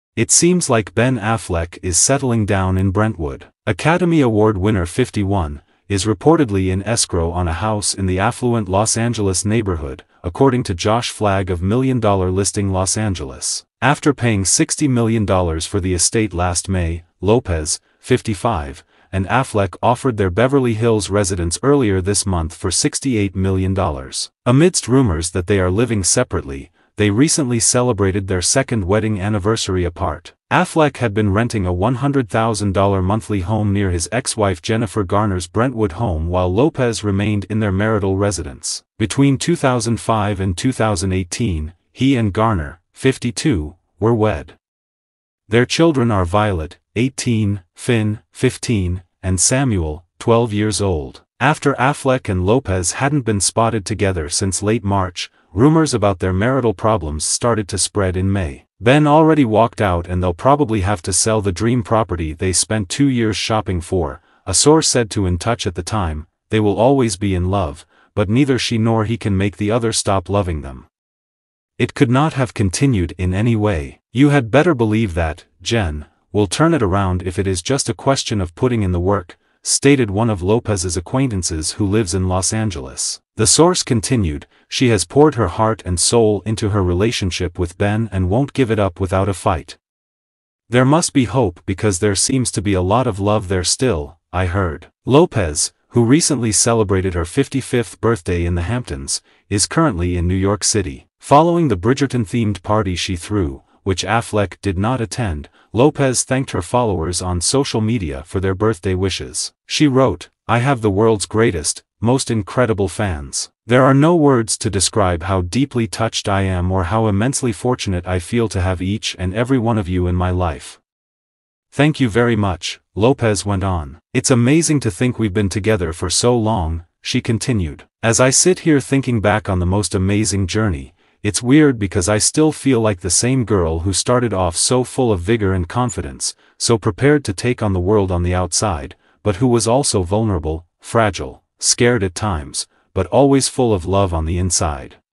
"Ben Affleck & JLo: Separate Lives?"
"Ben Affleck and Jennifer Lopez's relationship faces challenges as reports suggest they're living in separate homes. Ben is in escrow on a new house in Brentwood, while JLo remains in their Beverly Hills mansion. Amid rumors of marital woes, both stars are focusing on their individual lives. Could this be the end of Bennifer, or is there hope for reconciliation? Discover the latest updates and insights into their complex love story."